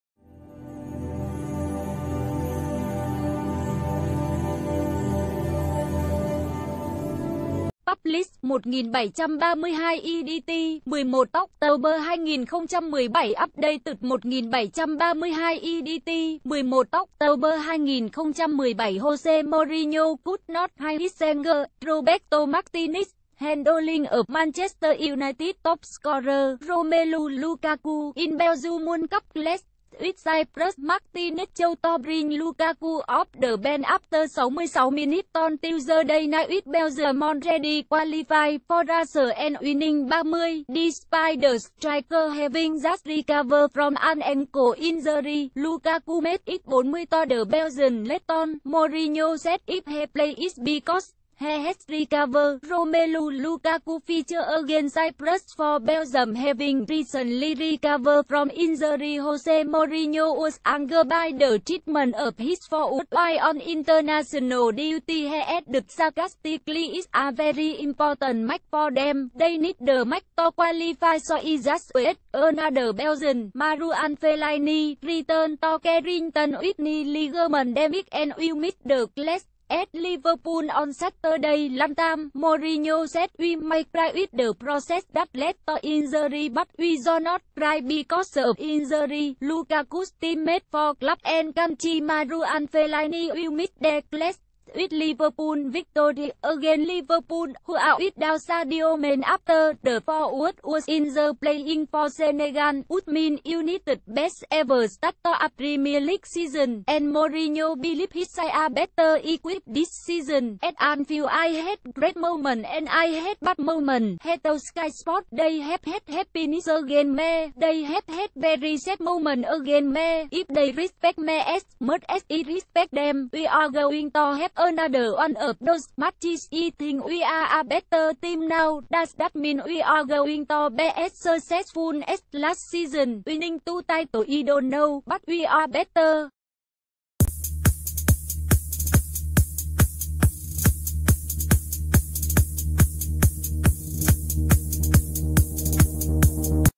[NVC] Mourinho angry after lukaku was risked in belgium's clash with cyprus
Mourinho angry after lukaku was risked in belgium's clash with cyprus